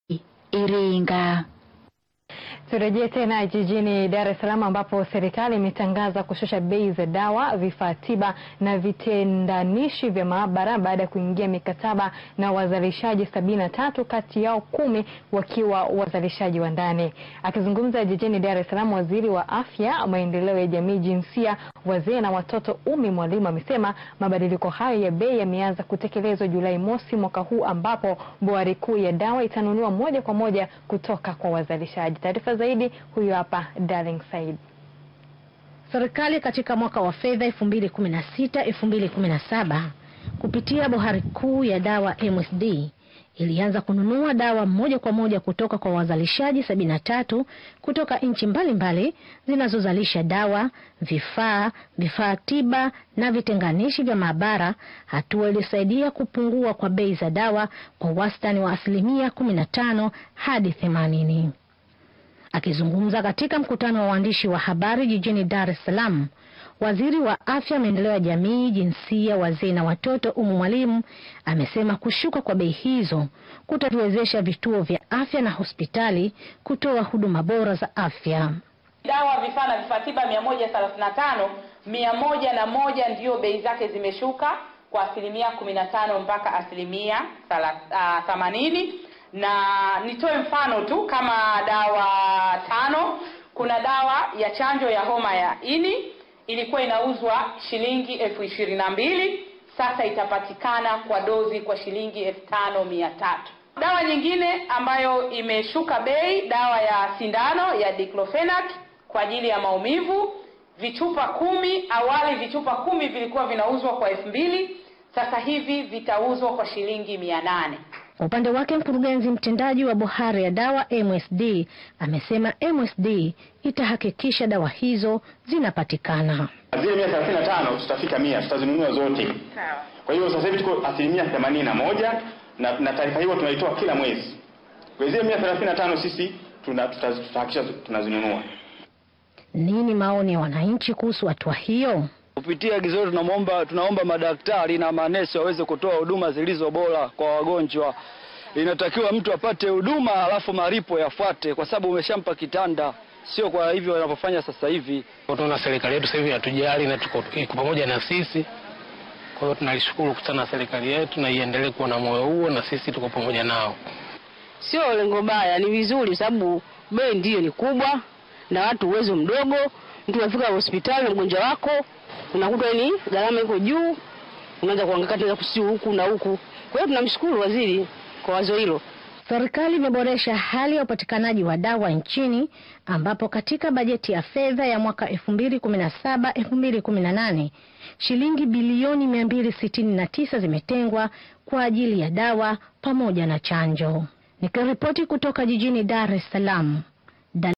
Serikali Yashusha Bei za Dawa na Vifaa Tiba
Serikali ya Tanzania imetangaza kushusha bei za dawa na vifaa tiba baada ya kuingia mikataba na wazalishaji 73 wakiwemo wazalishaji wa ndani